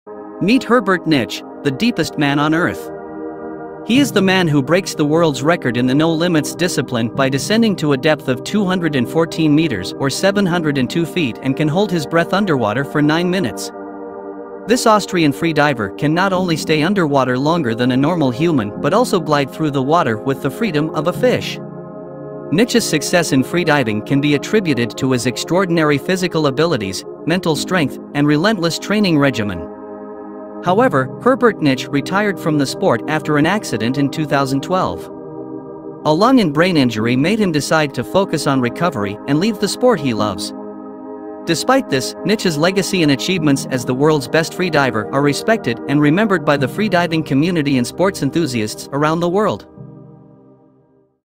The Deepest Man on Earth.
Herbert Nitsch (born 20 April 1970) is an Austrian freediver who has held world records in all of the eight freediving disciplines recognised by AIDA International. He is the current freediving world record champion and "the deepest man on earth". This title was given to him when he set a world record in the "No Limit" discipline at the depth of 214 meters (702 feet).